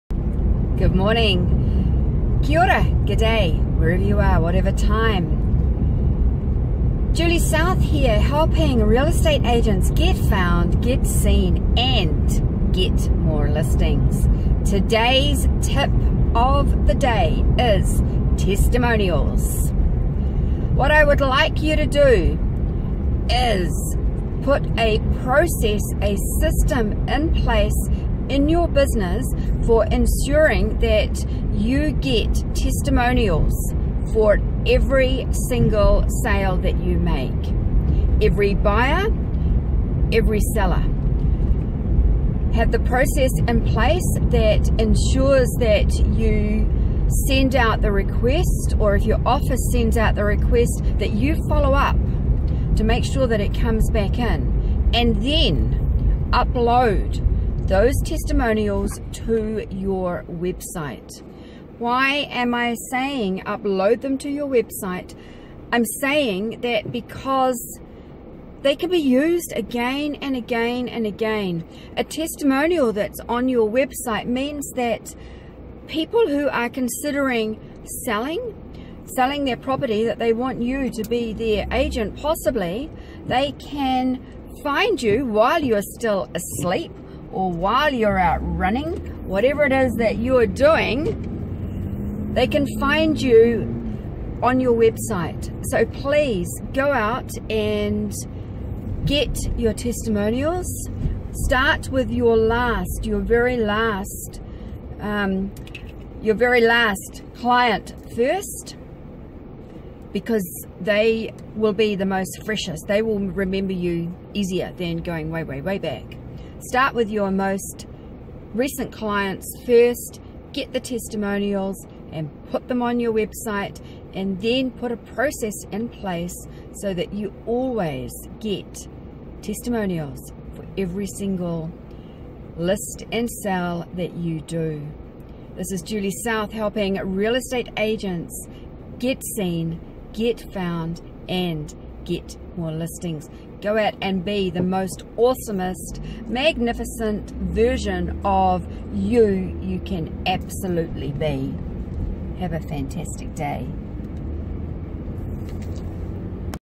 Real Estate Agents - testimonials - importance of having a system
watch this short video to discover the importance of having a systemised plan of action regarding client testimonials.